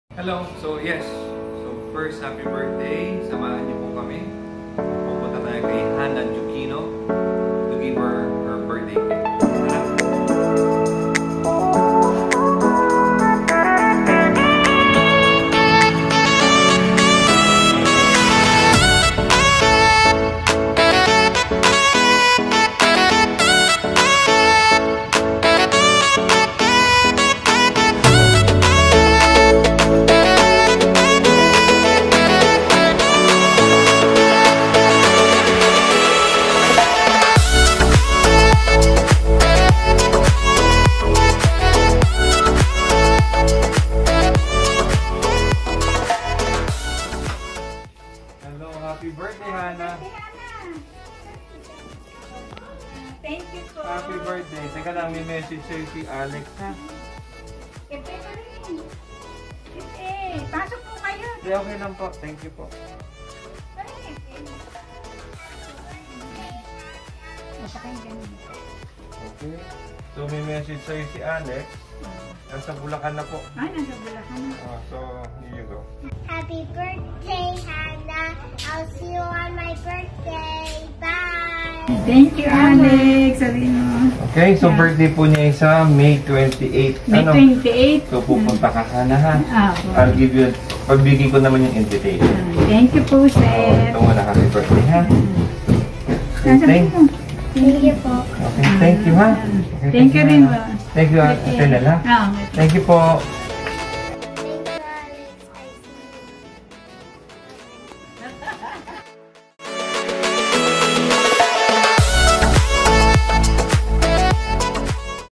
UNISAN QUEZON | Cake Para Sa Ating Mga Kids | Hannah Dioquino Brgy. R. Lapu-Lapu | MNP Sonny Puache
HAPPY BIRTHDAY SA ATING MGA KIDS NG UNISAN, QUEZON!

Ate Hannah Dioquino ng Brgy. R. Lapu-Lapu, Happy Birthday! Praying for your good health, to more blessings and more birthdays to come.

Kasama po si Baby Alex ay nagbigay tayo ng simpleng birthday cake kay ate Hannah!

Atin pong ipagdiwang ang kaarawan ng mga bata kahit sa simpleng paraan man lamang.

Maraming salamat po sa mga bumati ng happy birthday kay Baby Alex noong April 6, 2022.